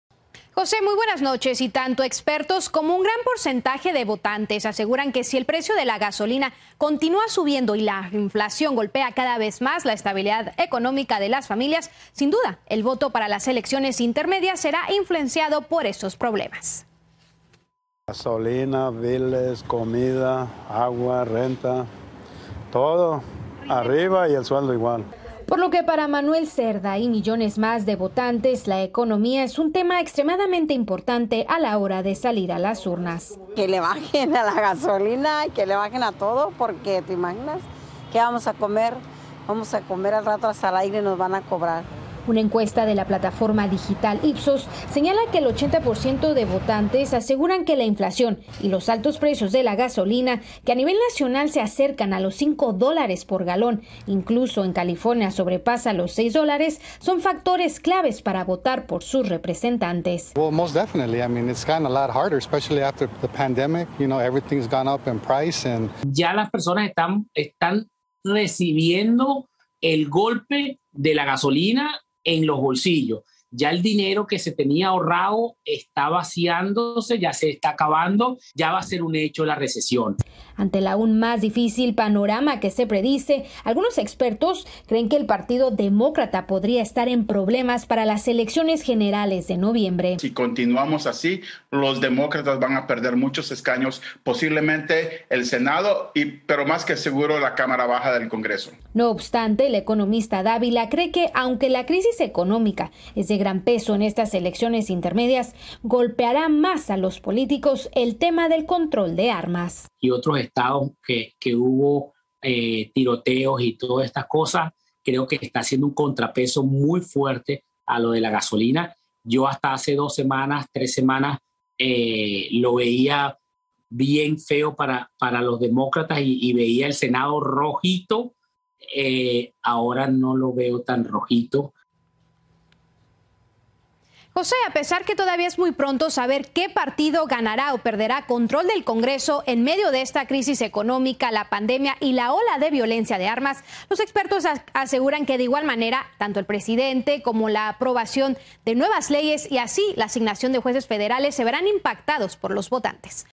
Inflación y votaciones en EE.UU. | Cierre de Edición
¿La economía que actualmente vive EE.UU. se verá reflejada en las decisiones de las urnas de votación? “Hasta el aire nos van a cobrar”, asegura una ciudadana ante los altos costos de productos como la gasolina.